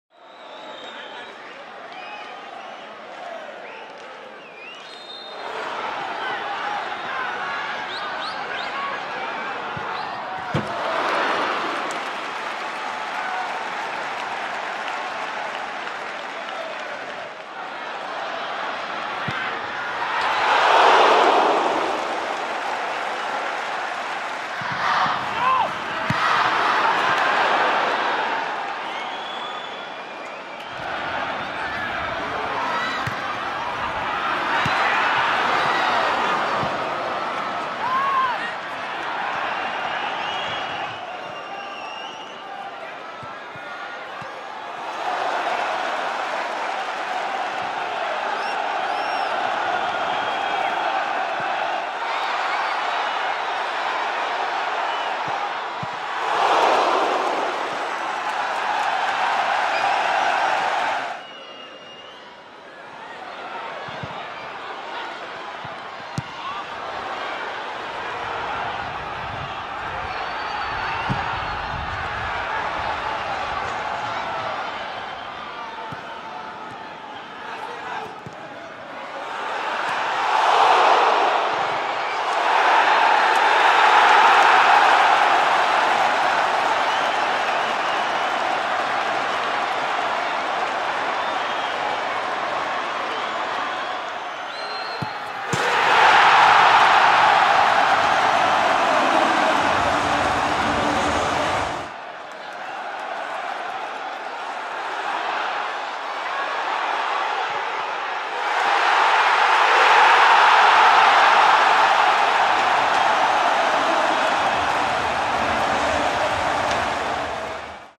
ADO Den Haag 2-0 Utrecht - Highlights - Round 30
A Roland Alberg penalty and a late strike by Gervane Kastaneer ensured ADO took all three points to move above Utrecht into 13th place in the Eredivisie.